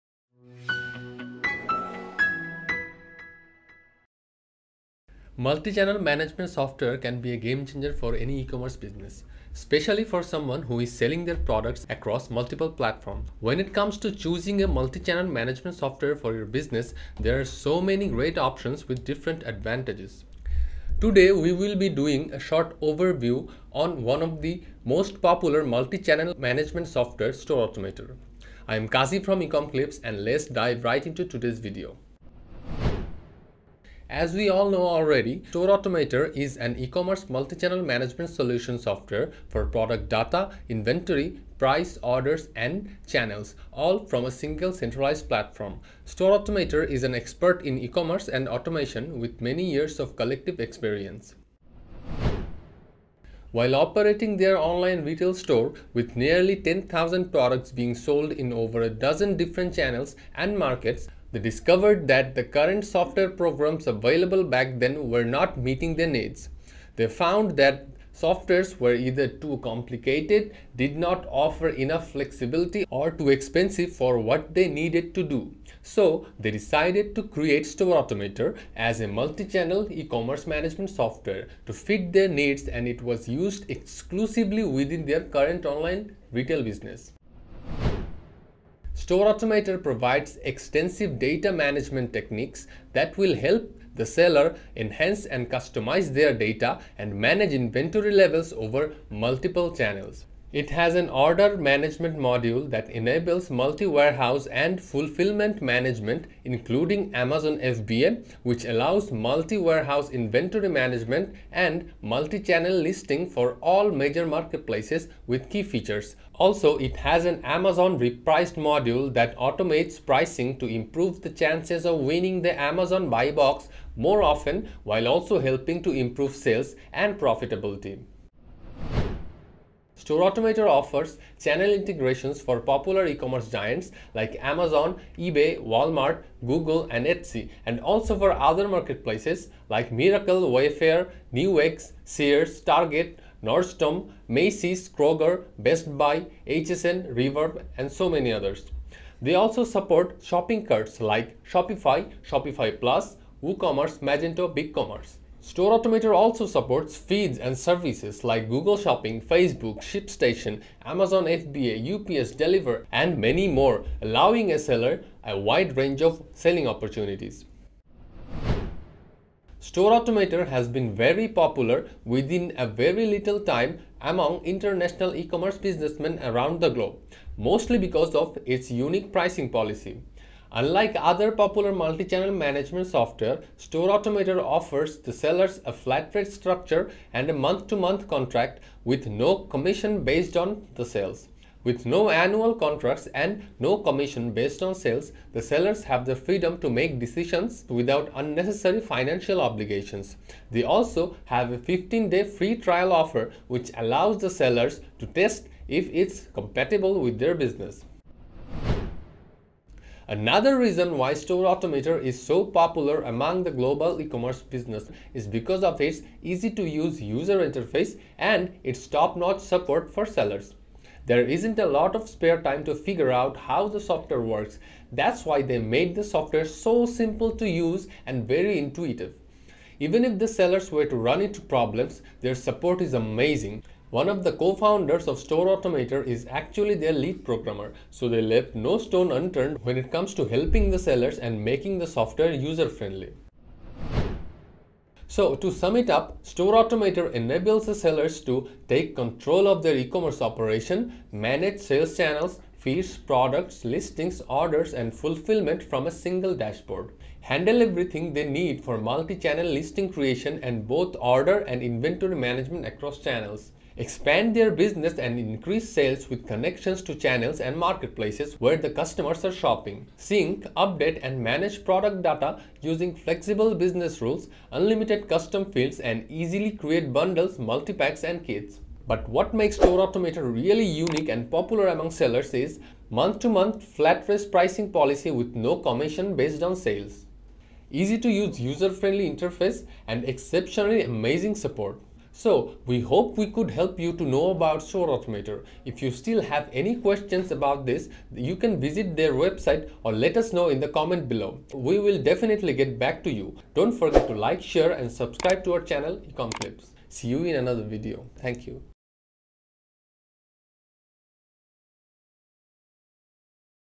StoreAutomator - A Complete 3rd Party Multi-Channel Inventory & Listing Management Software Overview
Multichannel management software can be a game-changer for any e-commerce business, especially for someone who is selling their products across multiple platforms. When it comes to choosing a multi-channel management software for your business there are so many great options with different advantages. One of them is StoreAutomator.

StoreAutomator is an e-commerce multichannel management solution software for product data, inventory, price, orders, and channels - all from a single centralized platform. StoreAutomator is an expert in eCommerce and Automation with many years of collective experience.

In today’s video, we have given a brief overview of one of the most popular multi-channel management software, Storeautomator.

We focused on these below topics:

✅ How StoreAutomator Started

✅ What It Provides

✅ Marketplaces, Shopping Carts, and Others they work with

✅ Pricing Policy

✅ Easy User Interface and Support

✅ Why Choose StoreAutomator

We hope we could help you to know about Storeautomator through this video session of Ecomclips.

If you require any additional assistance, please leave a comment in the comment section, and we will get back to you with the proper solution.

Looking forward to seeing you in the next video.